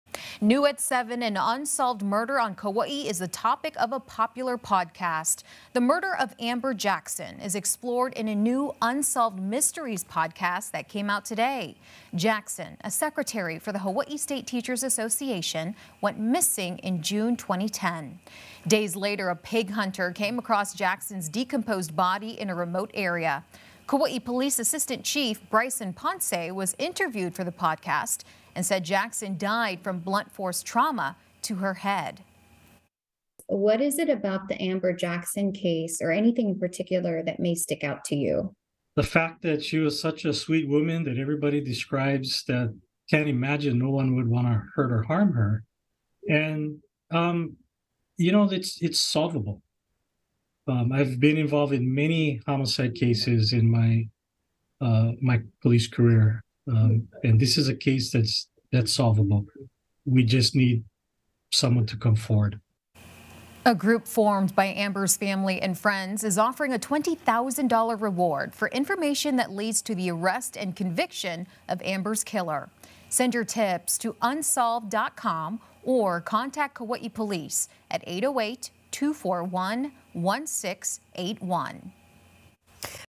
The unsolved murder of Kauai woman Amber Jackson featured in new Unsolved Mysteries podcast
A Kauai unsolved murder case is featured in the popular Unsolved Mysteries podcast.